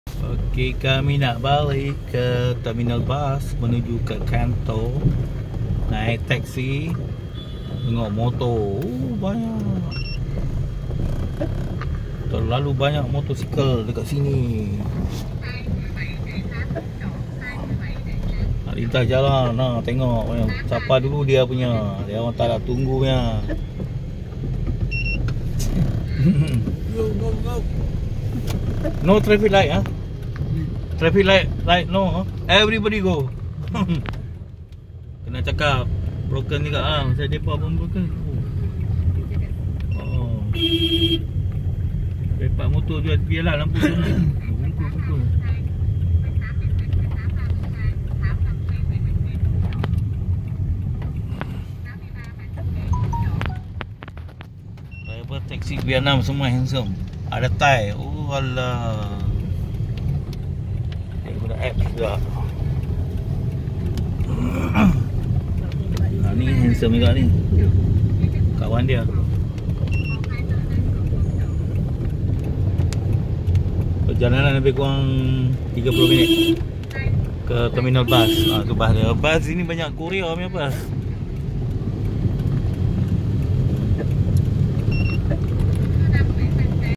Ho Chi Minh ke Can Tho naik bas Sleeper
Dari Hotel naik Vinasun Teksi